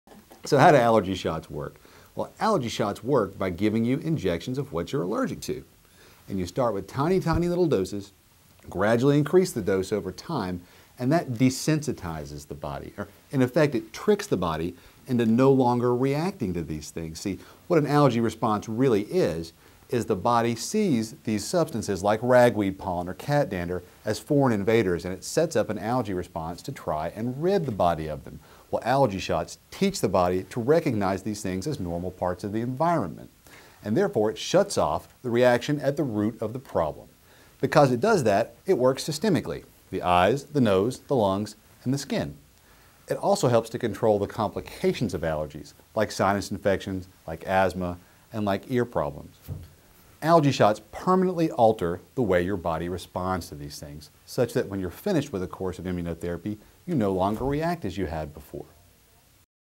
Allergy Shots: How Do They Work?
Board certified allergist Dr. John Overholt describes how allergy shots can potentially cure your allergies. For more educational videos on allergies and asthma,